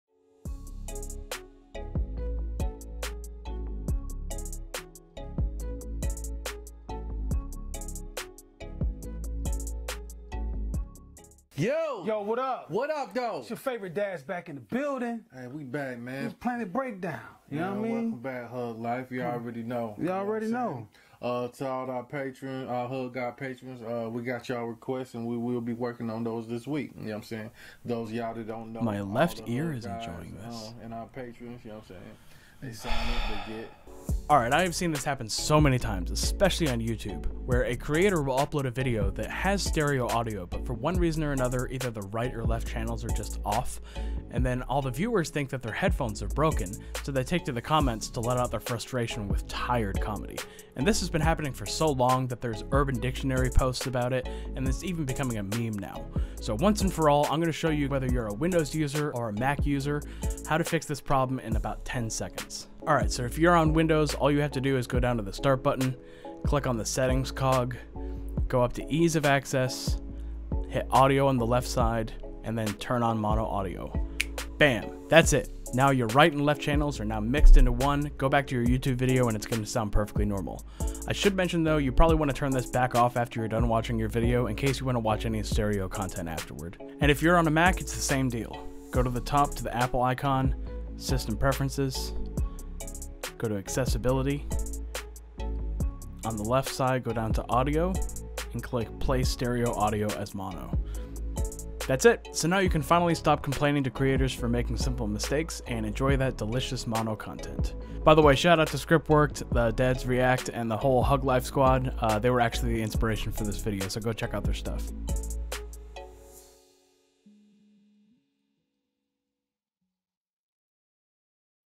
Fix YouTube Audio Only In One Ear
Videos often get uploaded to YouTube with stereo sound but only on one channel. So much so that it's becoming a meme to make cheezy jokes about it. "My left ear is enjoying this", "my right ear feels lonely", etc.

In this video, I show a quick workaround to mix stereo audio into mono on Windows and Mac OSX.